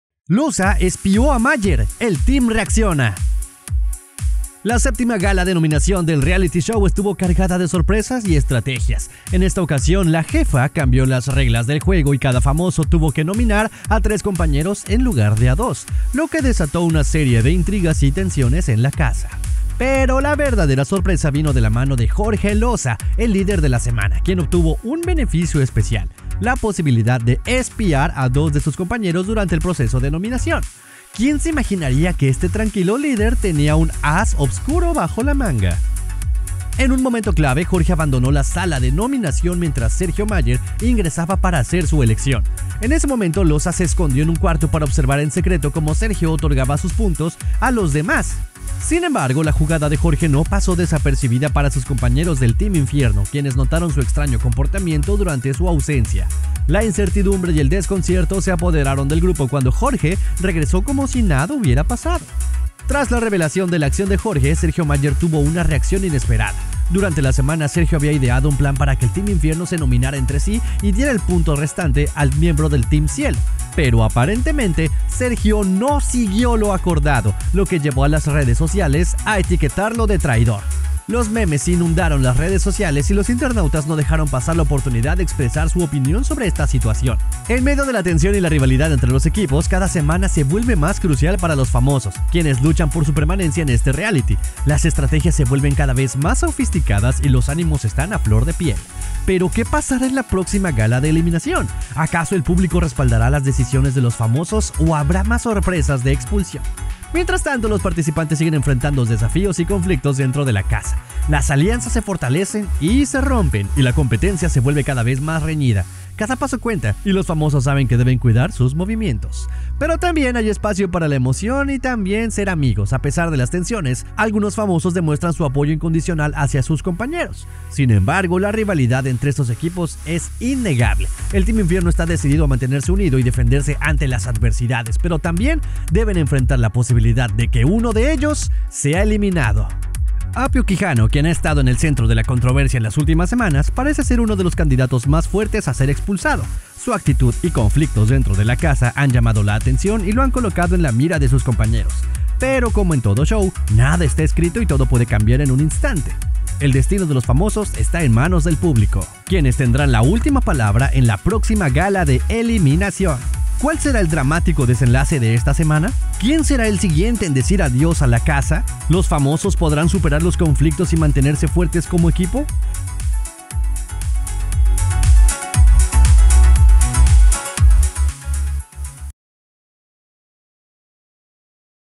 Jorge Losa espió a Sergio Mayer
Jorge Losa espió a Sergio Mayer durante la gala de nominaciones